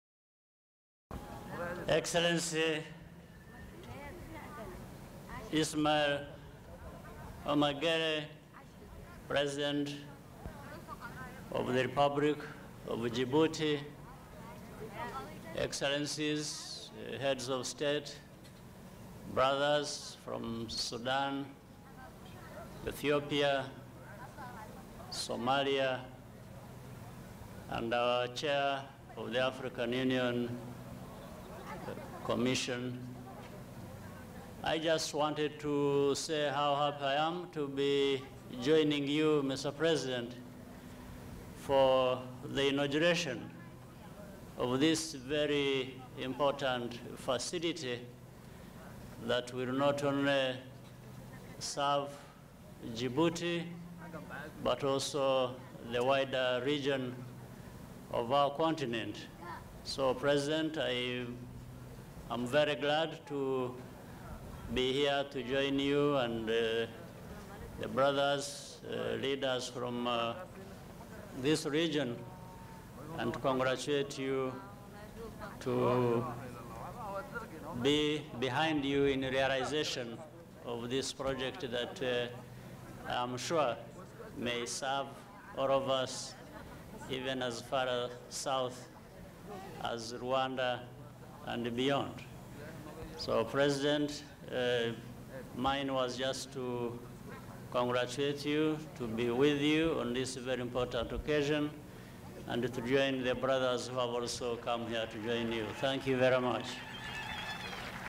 President Kagame speaks at the Inauguration of Djibouti International Free Trade Zone
Inauguration of Djibouti International Free Trade Zone | Djibouti, 05 July 2018